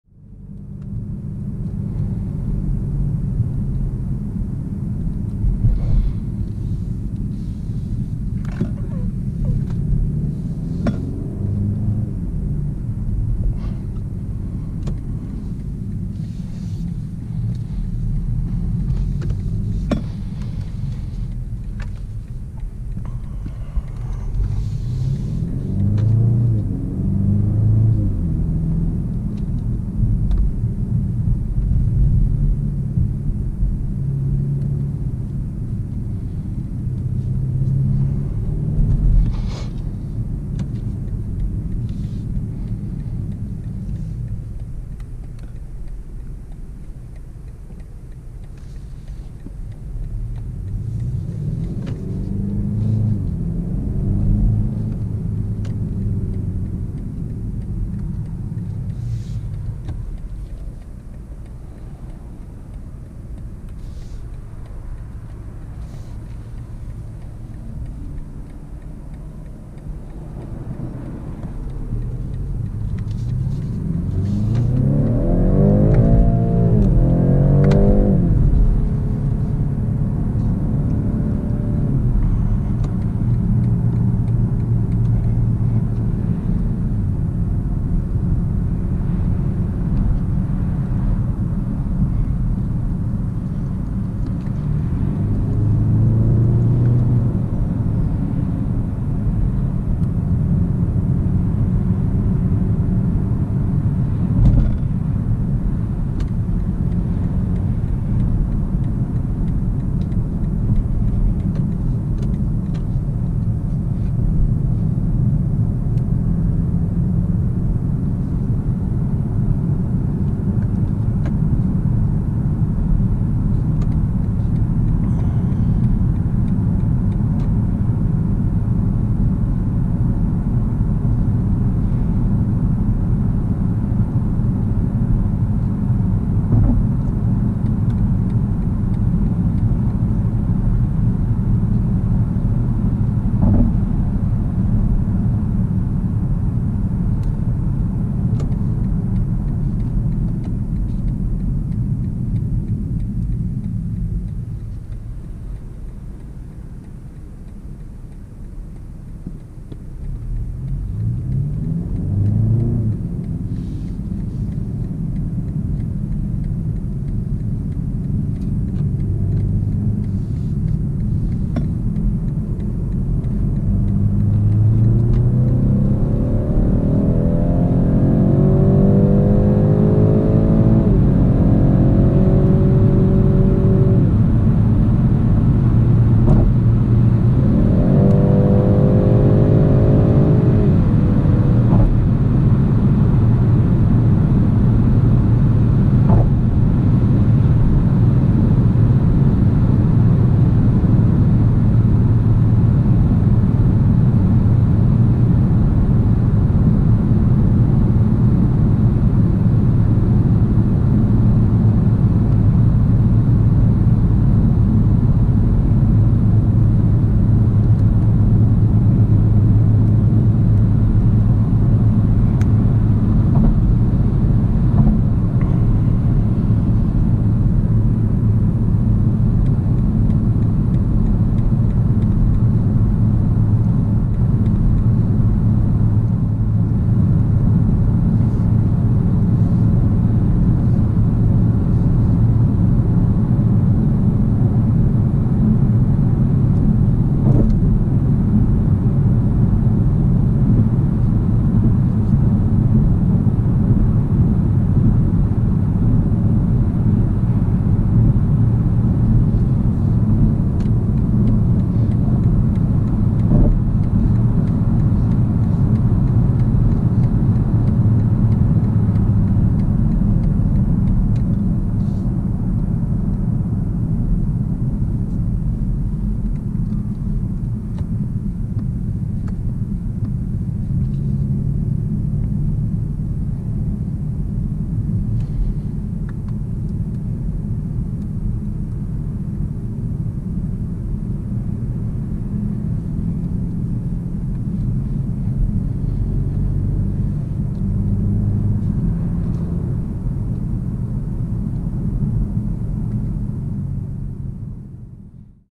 2021 BMW 530e xDrive POV Drive
0:00 Testing rear-wheel steering on narrow streets
1:08 Quick squirt of acceleration
1:20 Driving towards the motorway
2:53 Driving on the motorway